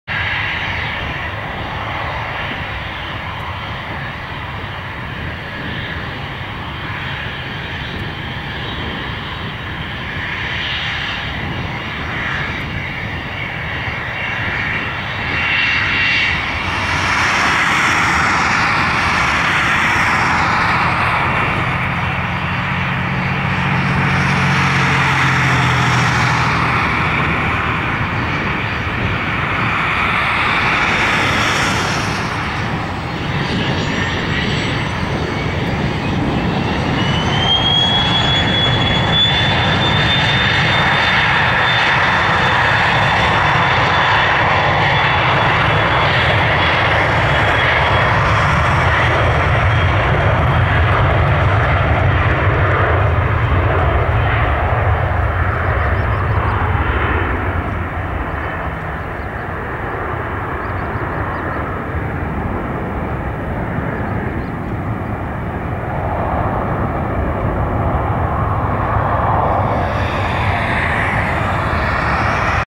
Cessna 560XL [N746PC] Takeoff Portland Airport (PDX)
Flight: N746PC PDX-SUN